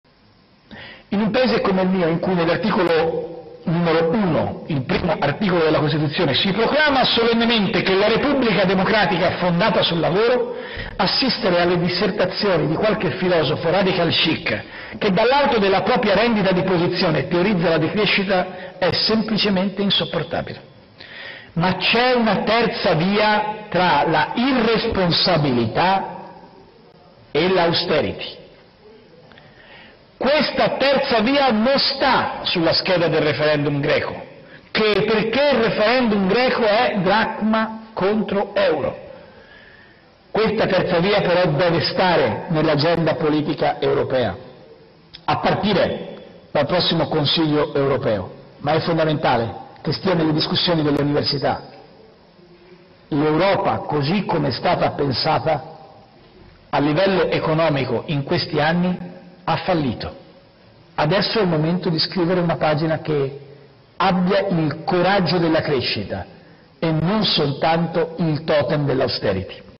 RENZI: C'E' UNA TERZA VIA TRA L'IRRESPONSABILITA DI TSIPRAS E L'AUSTERITA'
(Agenzia VISTA) - Berlino, 1 Luglio 2015 - Il Presidente del Consiglio Matteo Renzi all’Università di Humboldt / CHIGI